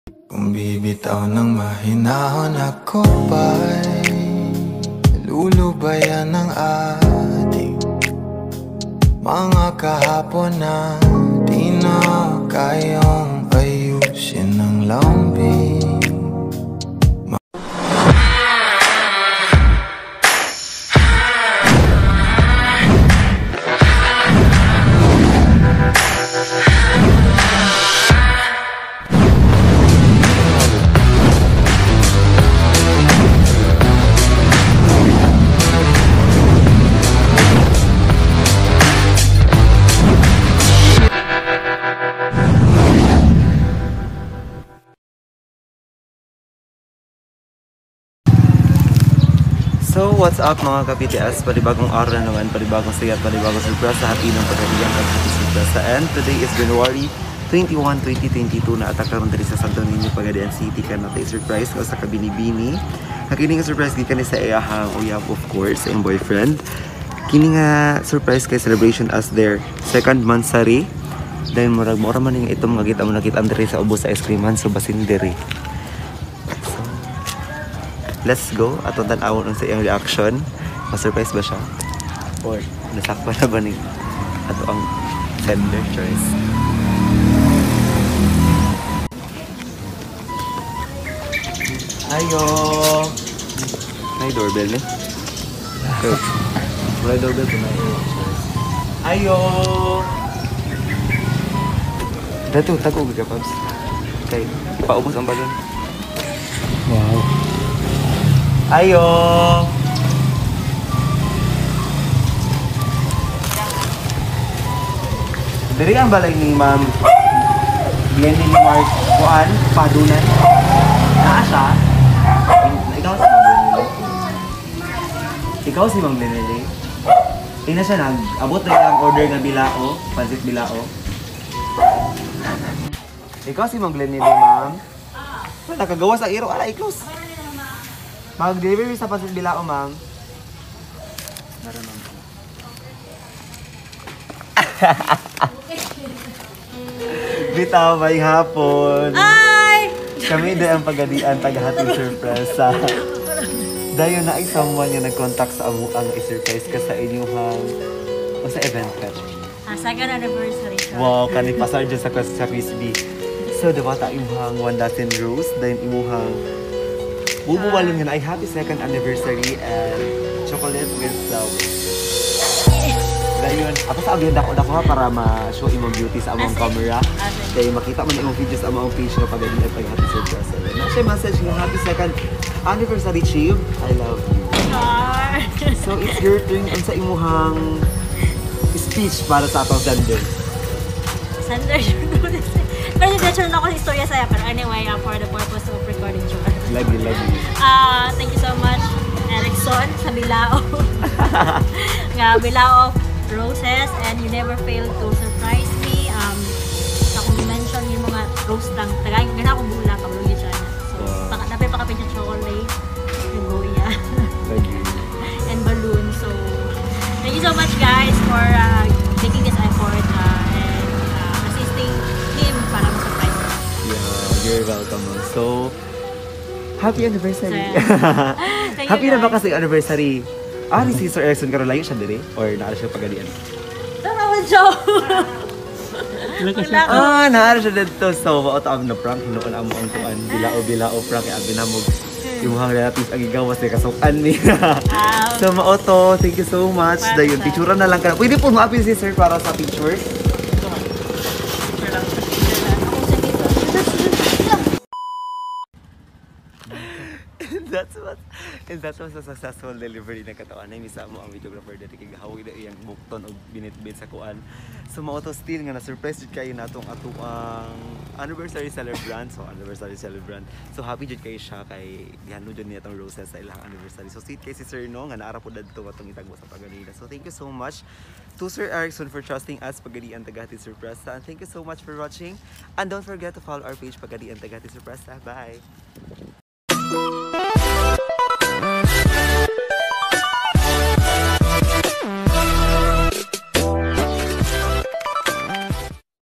You Never Fail to Surprise me! 😍🥰 / Pagadian Tagahatid Surpresa / Surprise Vlog
"You Never Fail to Surprise me!" 😍🥰
Dapat araw araw Nililigawan at Pinapakilig ang Partners Natin

Successful delivery at Sto. Nino, Pagadian City
Thank You for Trusting Team PTS!